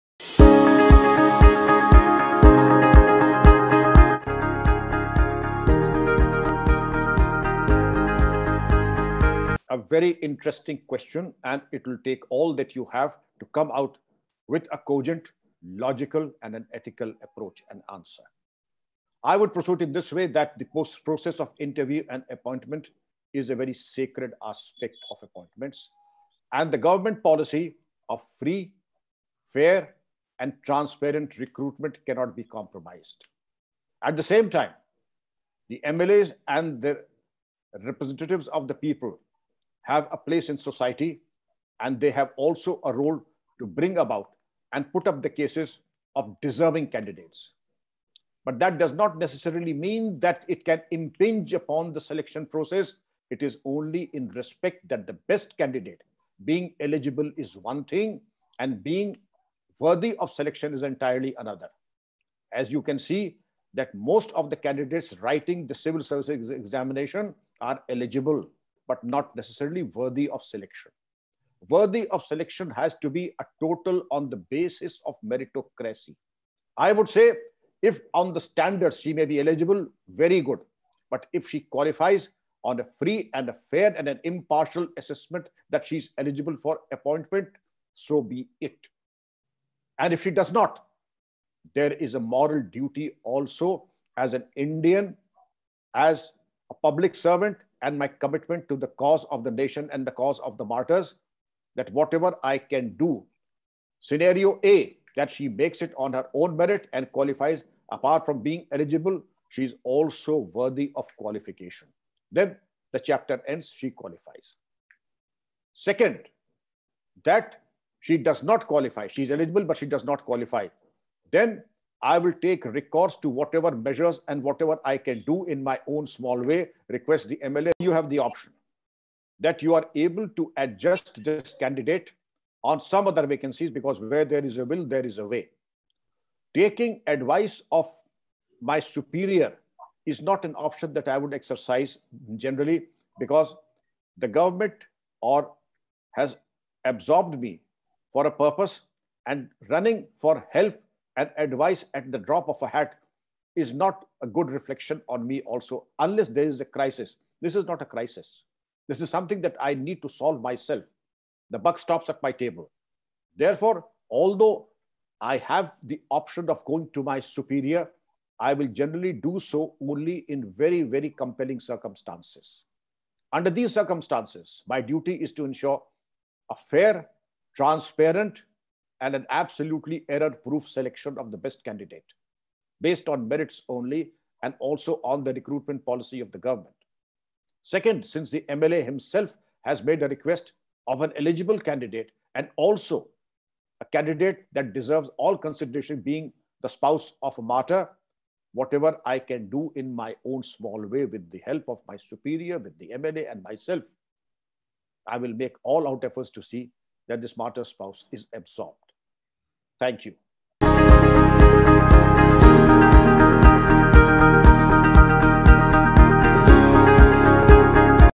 GS MAINS 2021। PAPER-4 । ETHICS AND INTEGRITY। Case Studies । Dr Vikram Singh, IPS. Ex. DGP (U.P.)
CASE STUDY
Case studies which test the candidate’s application of those concepts to situations involving the candidate and other stakeholders such as politicians, pressure groups, the public, and other people (125 marks).
In this Case study with Dr. Vikram Singh, IPS Retd. Ex. DGP (U.P.) & Chancellor NIU has given his perspective to solve the situation in ethical way.

Best Online Coaching for Civil Service_IAS_ UPSC_IFS_IPS, Free Study Material ENSEMBLE IAS ACADEMY